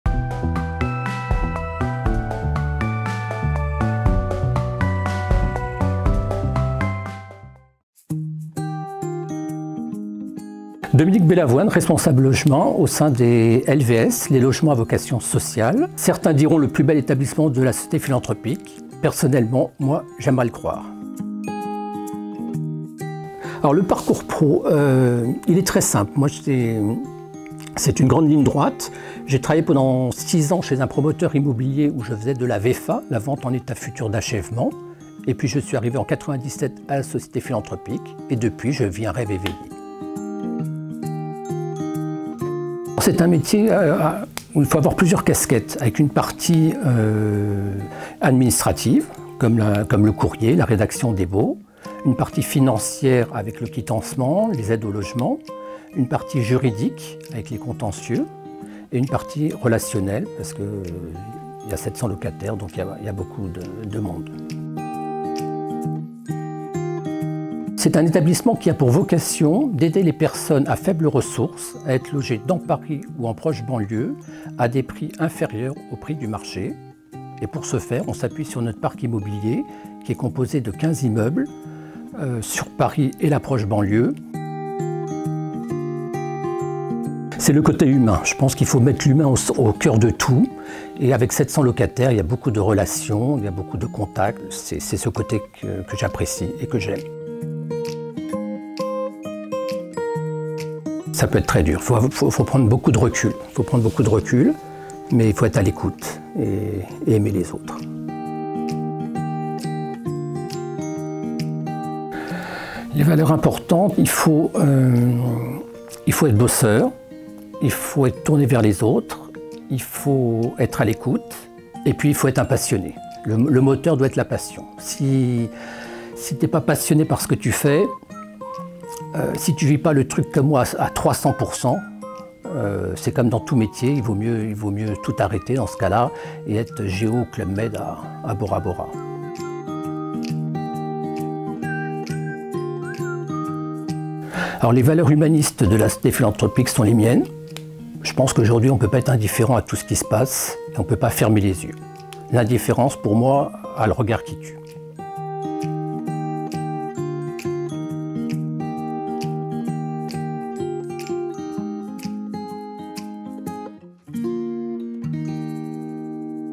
Philm' & Moi avec Dominique - Episode N°7
Philm' & Moi est une série de courtes vidéos mettant en lumière des professionnels et des personnes accompagnées au sein de la Société Philanthropique.

Ces vidéos laisseront donc place aux ressentis des employés et des personnes accompagnées.